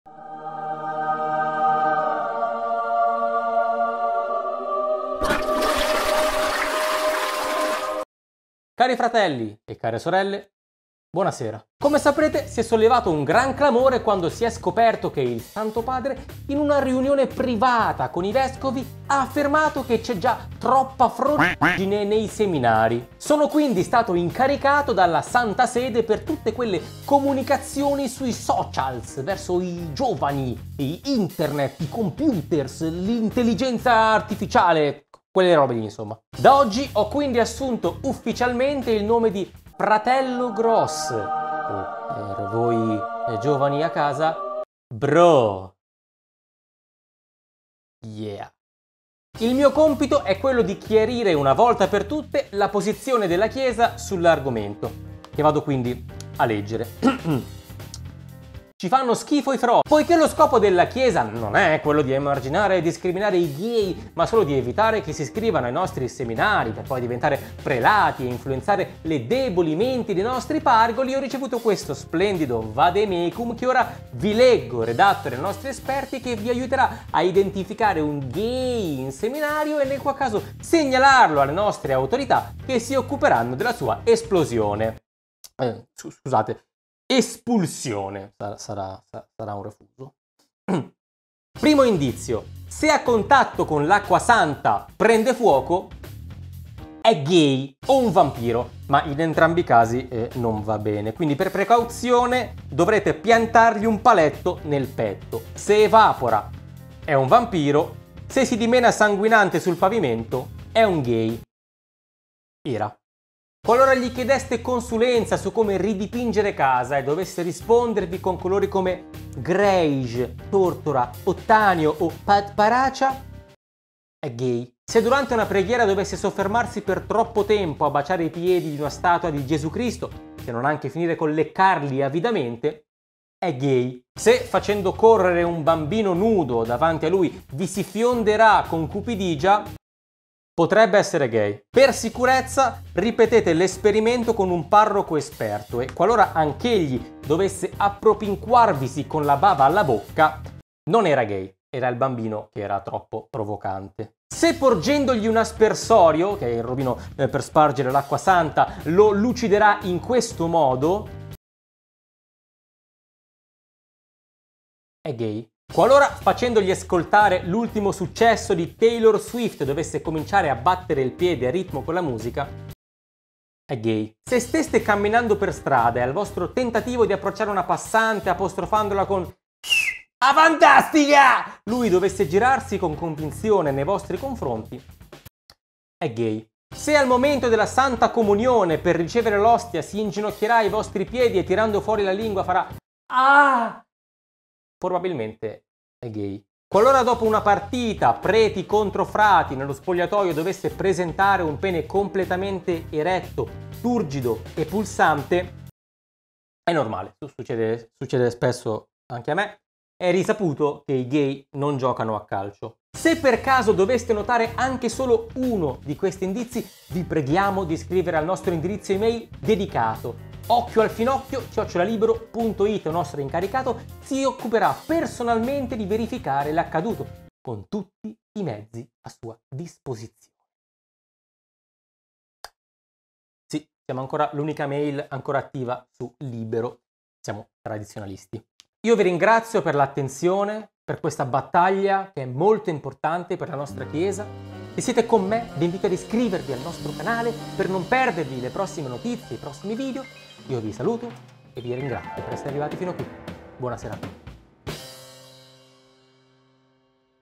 Il vademecum del Papa contro la froc***gine
Sono stato ufficialmente incaricato dal Santo Padre di occuparmi della comunicazione social della Santa Sede.
Il mio primo compito è quello di indicare chiaramente la via ai giovini fornendo tutti gli strumenti per diagnosticare il ghei ai seminaristi.